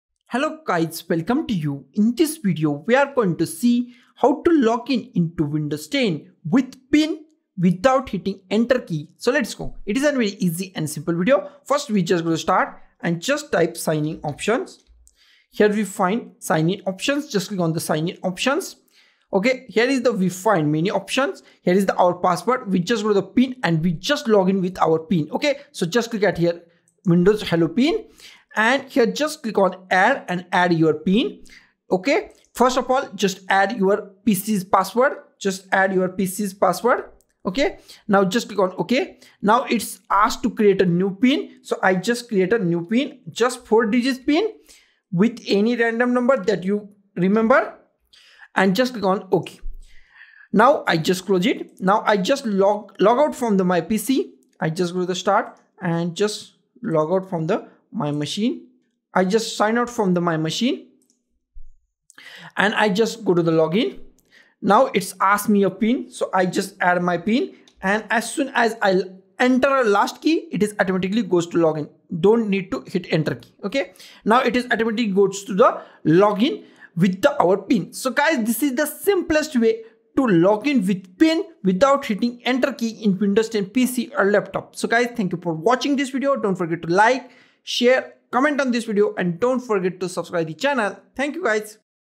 How to Login Windows 10 with PIN Without Hitting ENTER Key
Do you find it cumbersome to enter your PIN and then hit the ENTER key every time you log in to Windows 10? We have a solution for you! In this tutorial, we'll show you a quick and easy method to log in to your Windows 10 computer using a PIN without having to press the ENTER key.

Windows 10 offers PIN-based authentication as a convenient and secure way to access your computer. However, the additional step of pressing ENTER after entering your PIN can be time-consuming, especially if you frequently log in and out of your system.

In this comprehensive guide, we'll walk you through a simple workaround that eliminates the need to hit the ENTER key. With our step-by-step instructions, you'll learn how to configure Windows 10 to automatically log you in without any extra keystrokes.

By following this method, you'll be able to save time and streamline your login process, making it more efficient and hassle-free. Whether you're a beginner or an experienced Windows user, our tutorial is designed to be user-friendly and accessible.

Join us as we explore the Windows 10 settings and demonstrate how to enable the automatic login feature for PIN-based authentication. With just a few clicks, you'll be able to enjoy a seamless login experience without the need to press ENTER.

Don't let the small inconveniences slow you down! Watch our tutorial now and discover the simple trick to log in to Windows 10 with your PIN without hitting the ENTER key.